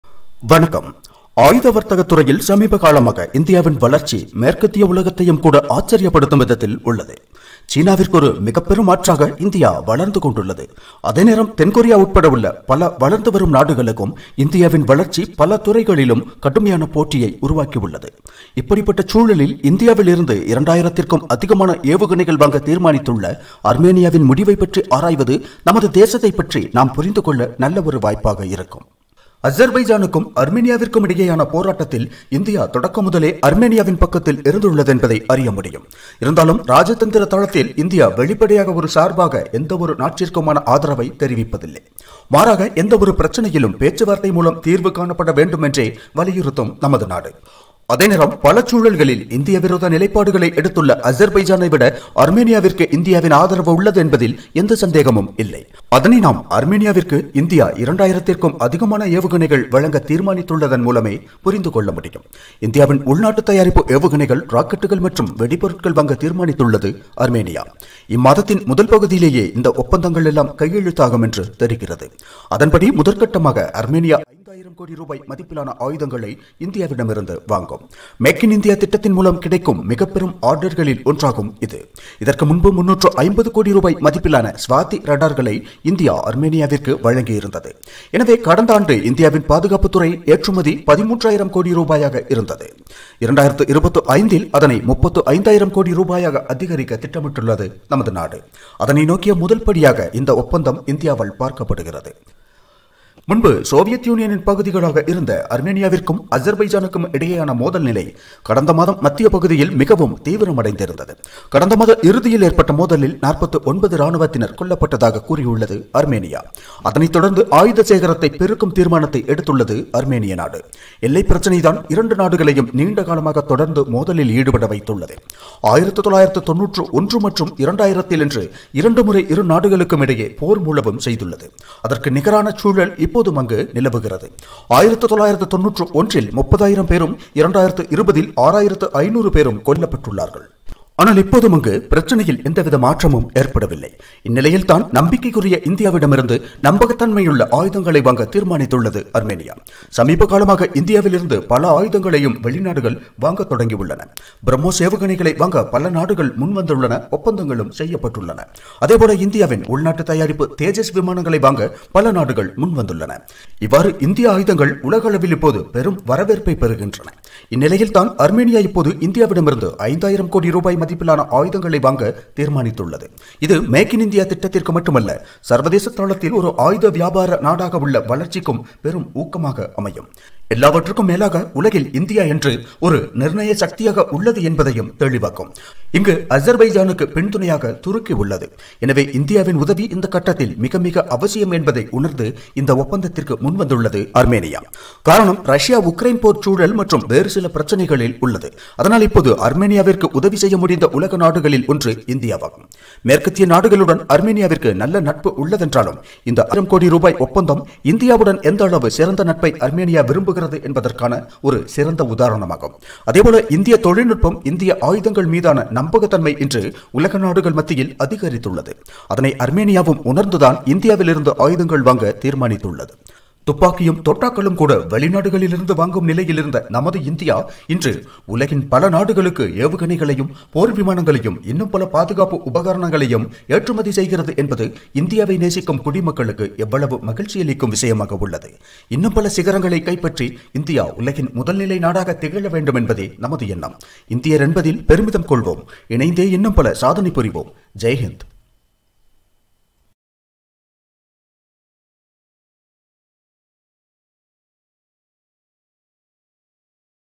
பல்லாயிரம் கோடி ரூபாய் ஆயுத ஏற்றுமதி, இந்தியாவின் எழுச்சி | India - Armenia | HF Tamil
Hello everyone

பல்லாயிரம் கோடி ரூபாய் ஆயுத ஏற்றுமதி, இந்தியாவின் எழுச்சி